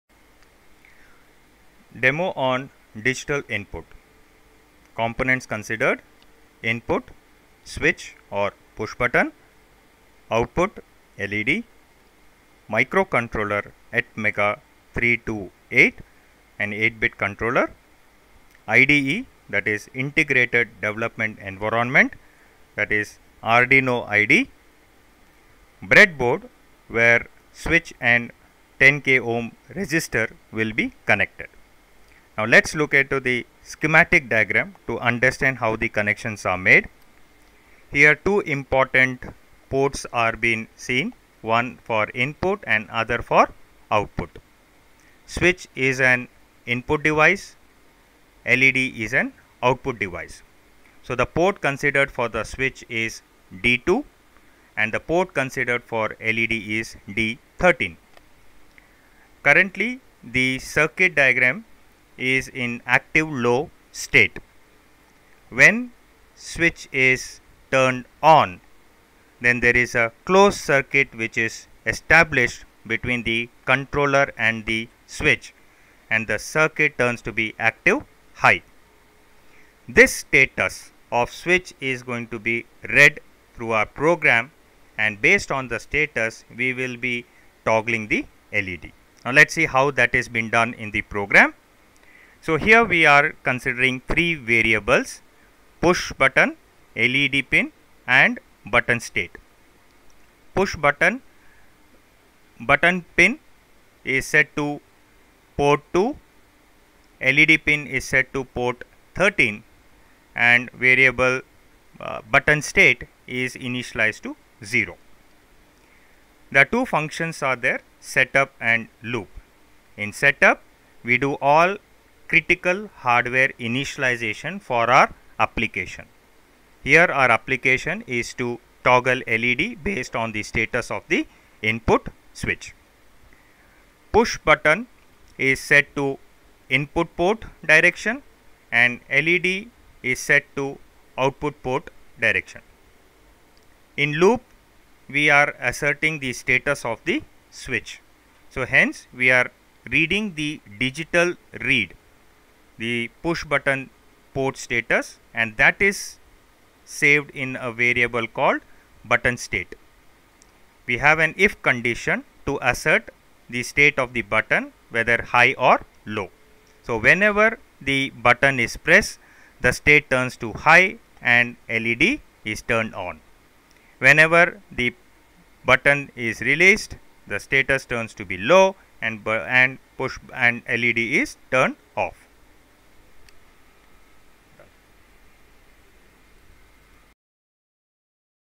Digital Switch
Learn about embedded systems, characteristic  and IPR and examples.